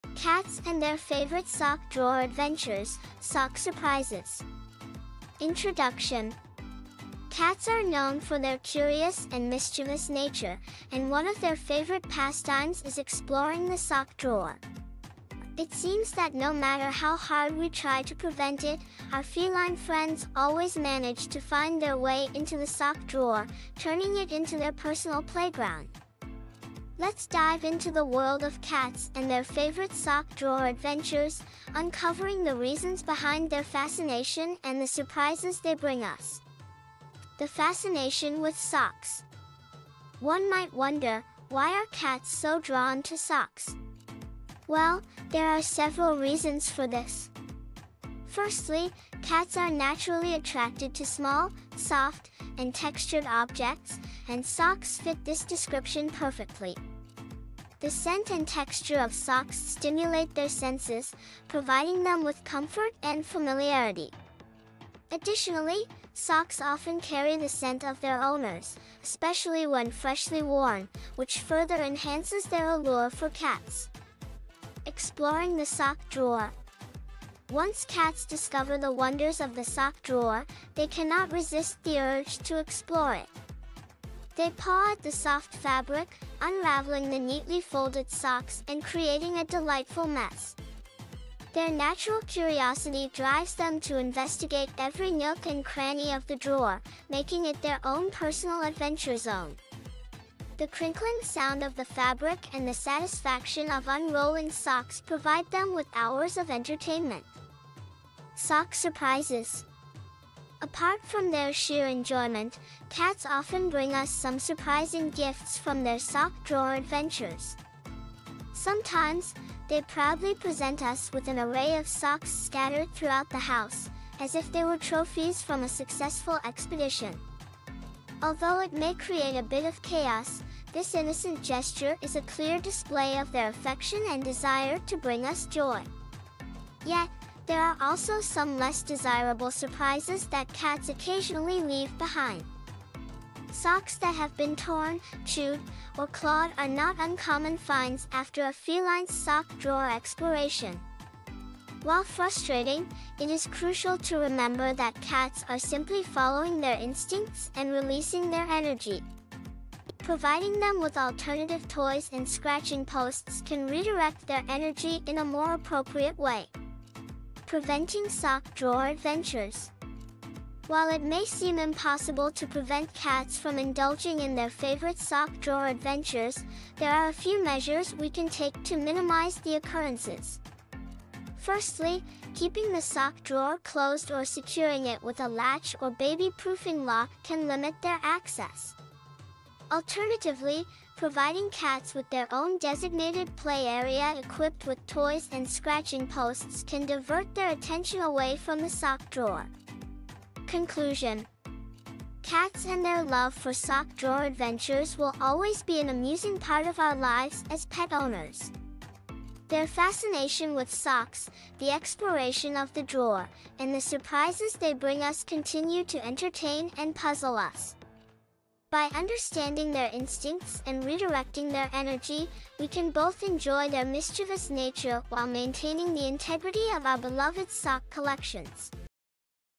Cats' Sock Drawer Adventure: Epic Sock Surprises!
Sock drawer adventures are a favorite pastime for cats, who love to explore and play in the comfort of their very own space. With a drawer full of surprise, cats can explore the various fabrics, colors, and patterns of socks, looking for the perfect hiding spot. Whether they are playing with socks, snuggling into a cozy pile of them, or just lounging amongst them, cats are sure to find plenty of excitement and comfort in their own little sock drawer world. Keywords: cats, sock drawer, exploring, fabrics, colors, patterns, hiding, playing, snuggling, lounging.